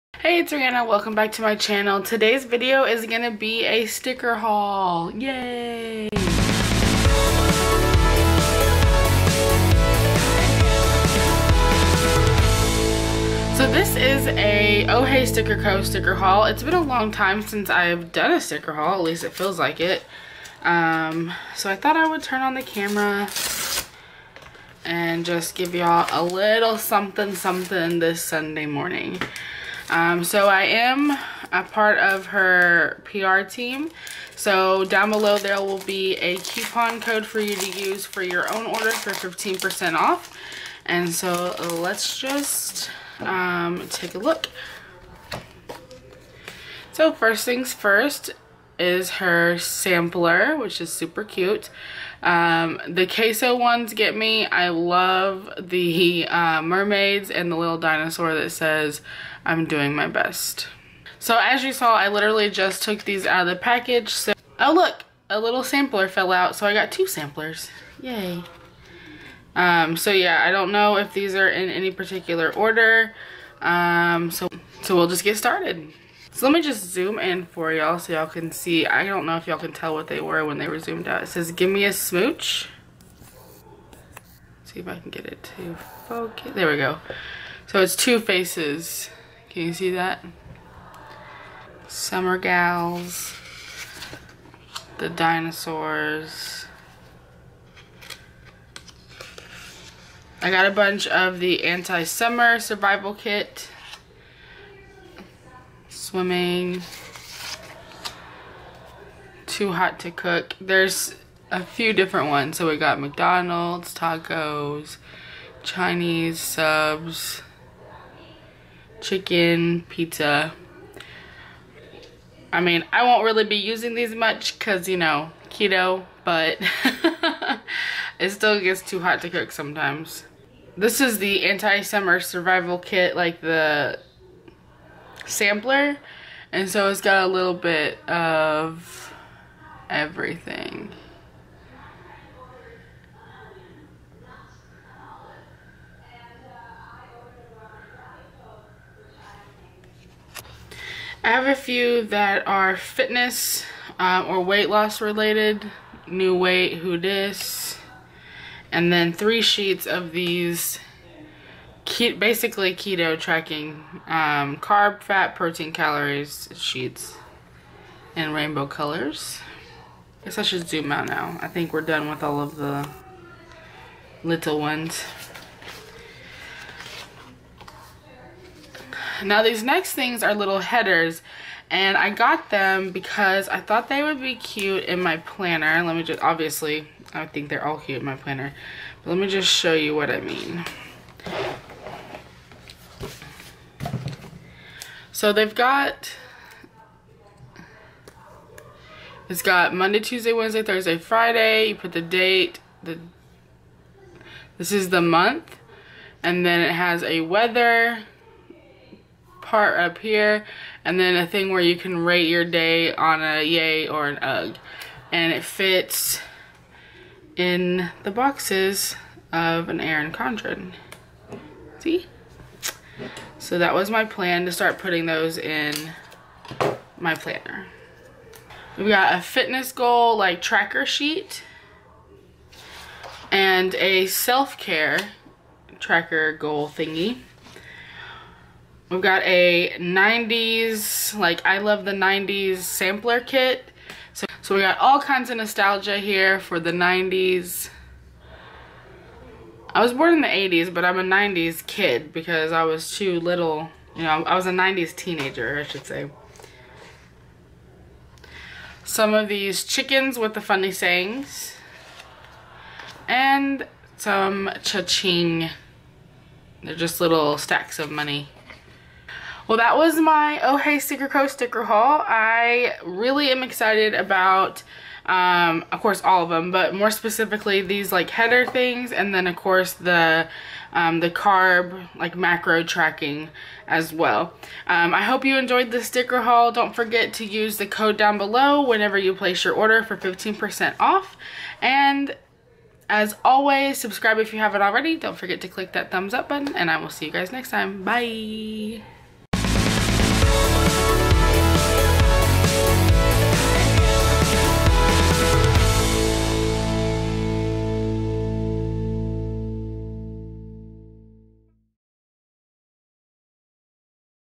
WILDCARD WEEKENDS | OH HEY STICKER HAUL
Hey girl hey! So many awesome new stickers, bc you can never have enough, em I right? lol

I would l love to have you be my friend.

Be sure to check out Oh Hey Sticker Co!